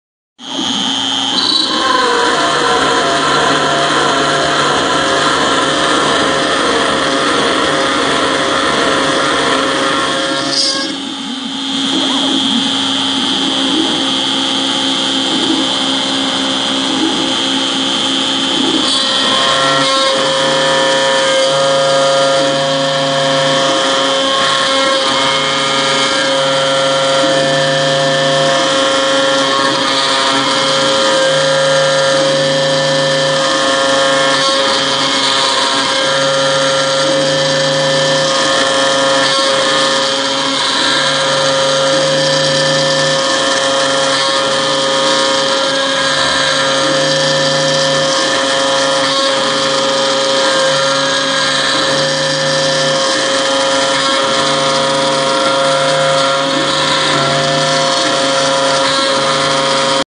Milling aluminum with MillRight CNC M3
Needed a spacer block. Was about 7 minutes from the point of "hey I need a 20mm wide spacer" to set it up in CAD/CAM and machine it out. You need one of these in your shop or garage.